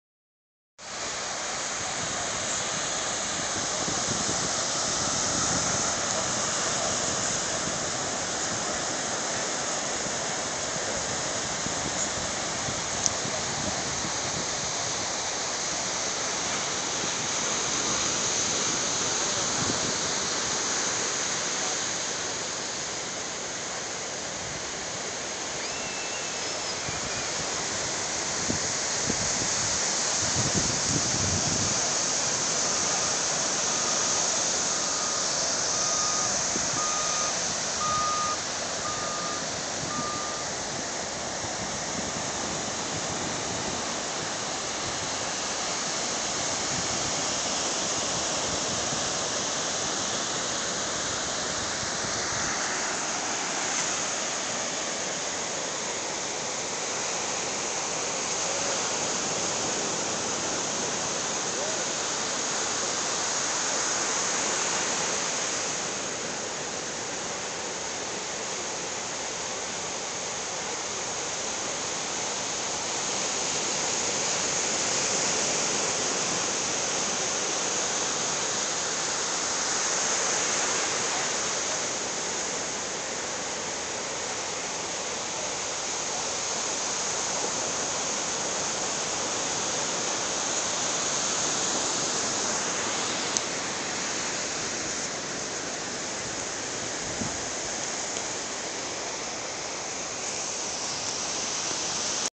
Younger my daughter surf at Santa Monica,California :)
first time instructor for surf